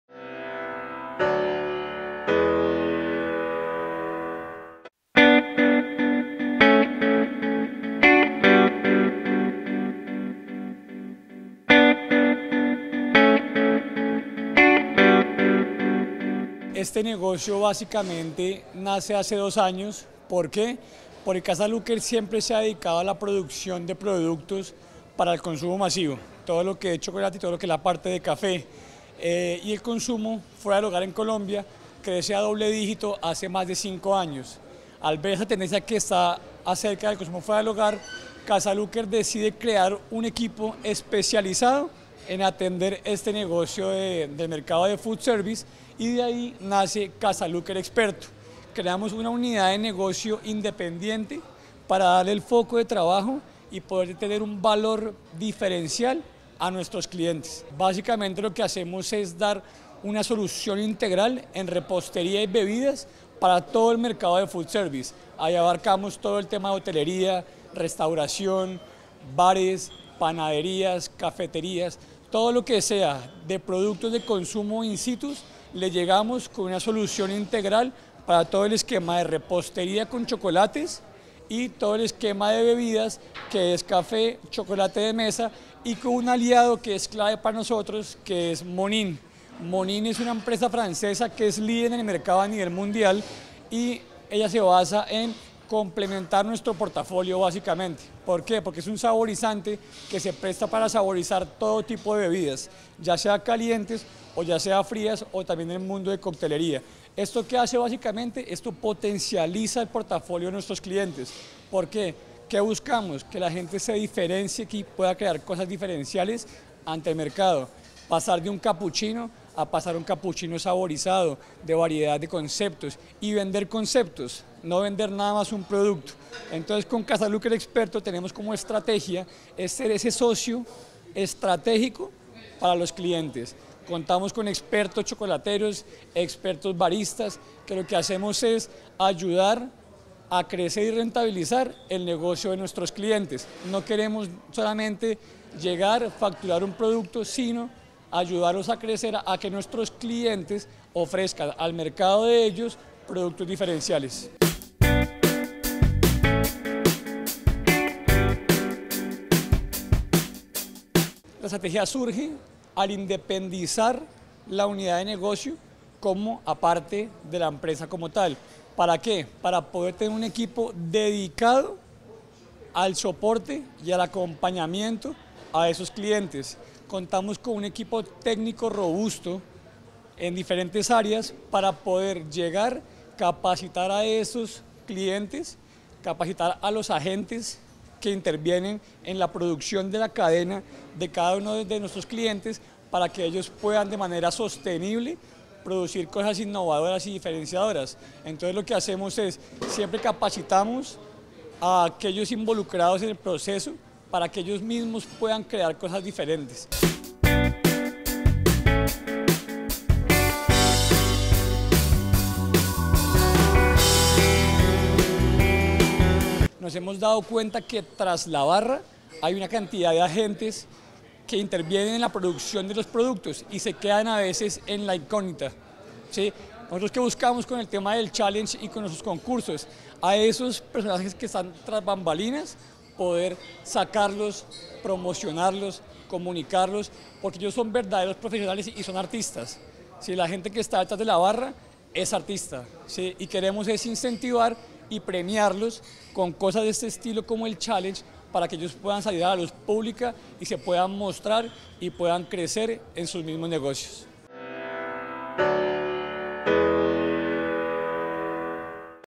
Casa Luker Food Service y el Challenge Colombia - [Negocios en Tm] ®
La empresaria y coach Internacional Paola Rueda López (@paolaruedalopez) conversó con Miguel Andrés Ángel, Director de Food Service para Casa Luker, sobre su incursión en el Food Service y el Challenge Colombia, un espacio creado para mostrar el talento de los bartenders.